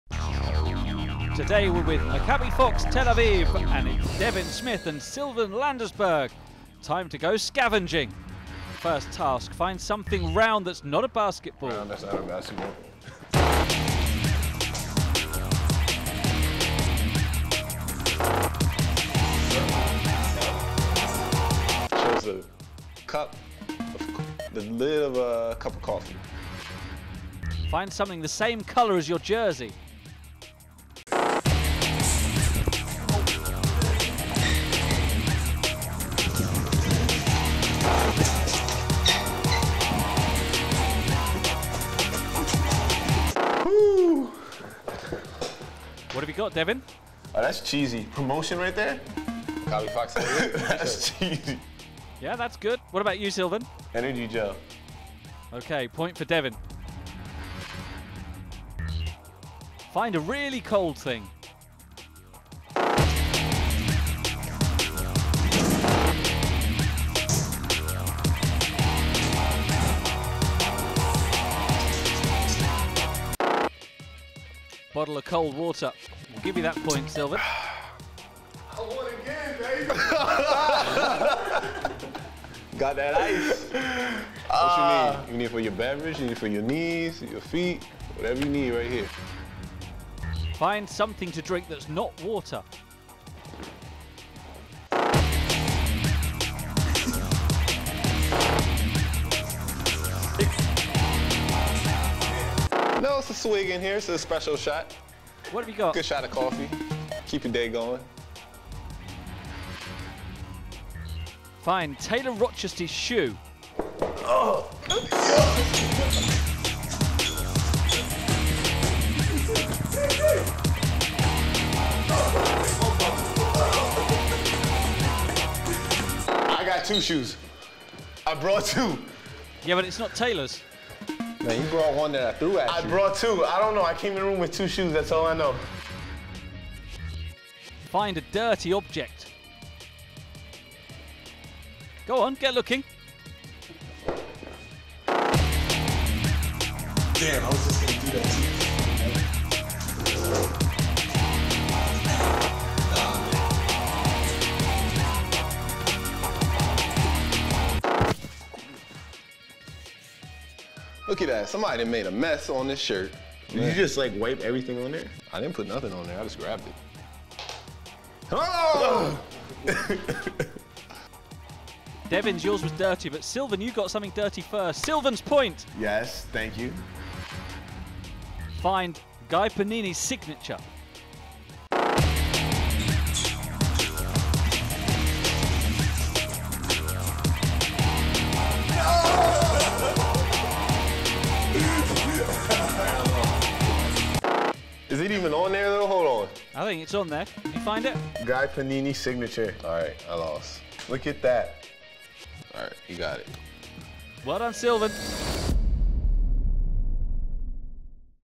Scavenger Hunt: Devin Smith and Sylven Landesberg, Maccabi FOX Tel Aviv
Today were with Maccabi FOX Tel Aviv Devin  Smith and Sylven Landesberg. Time to go scavenging.